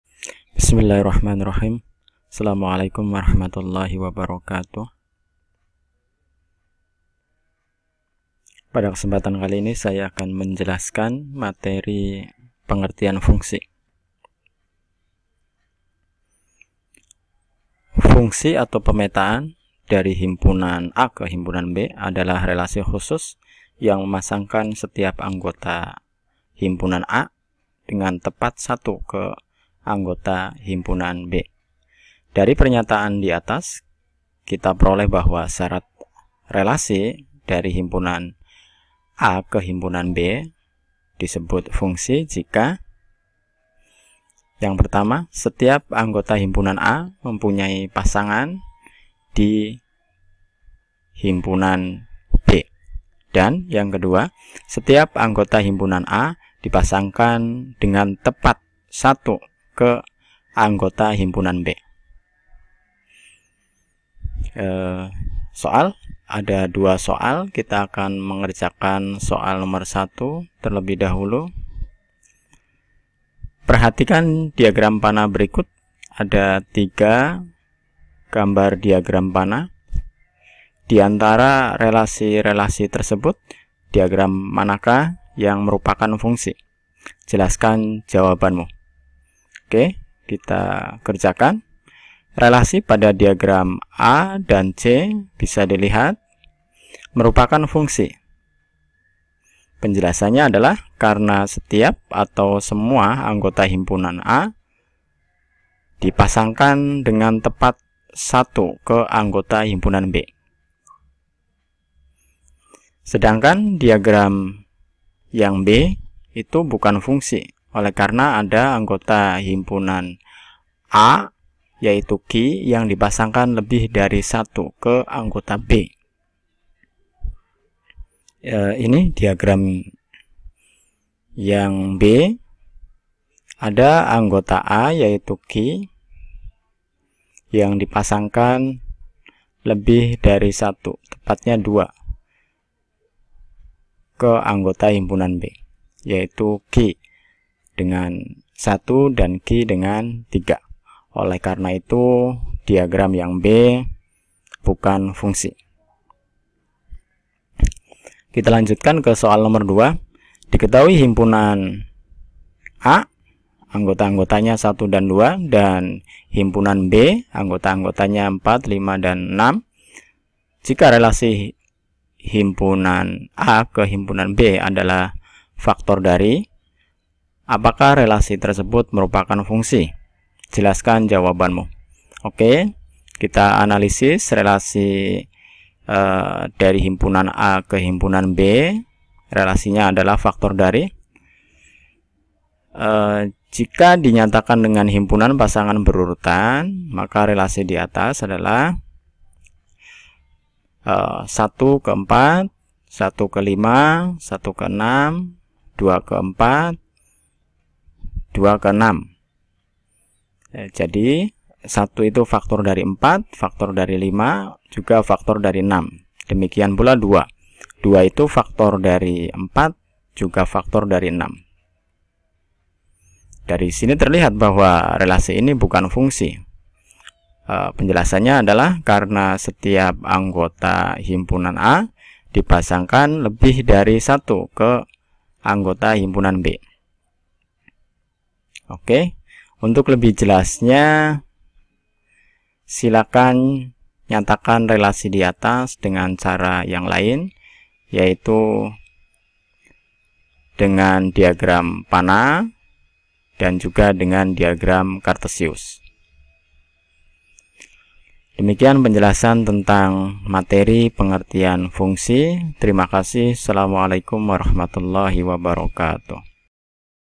Pengertian Fungsi
pada video pengertian fungsi di jelaskan pengertian fungsi dengan contoh soal relasi yang merupakan fungsi.